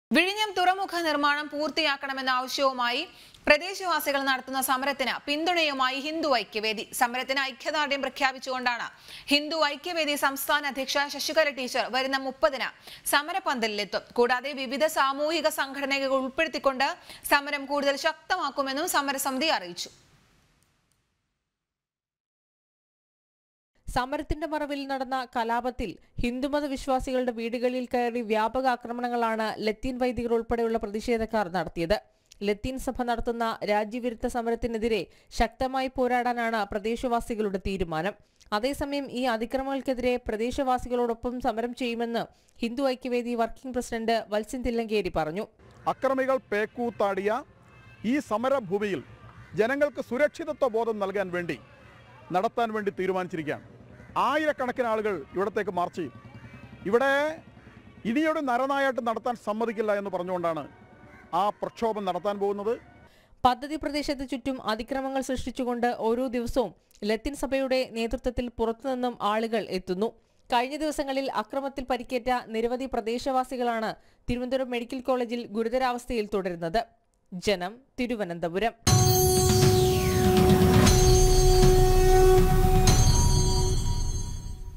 വിഴിഞ്ഞം തുറമുഖ നിർമ്മാണം; പൂർത്തിയാക്കാൻ പിന്തുണയുമായി ഹിന്ദു ഐക്യവേദി | JANAM TV
വിഴിഞ്ഞം തുറമുഖ നിർമ്മാണം; പൂർത്തിയാക്കണമെന്ന ആവശ്യവുമായി പ്രദേശവാസികൾ നടത്തുന്ന സമരത്തിന് പിന്തുണയുമായി ഹിന്ദു ഐക്യവേദി | JANAM TV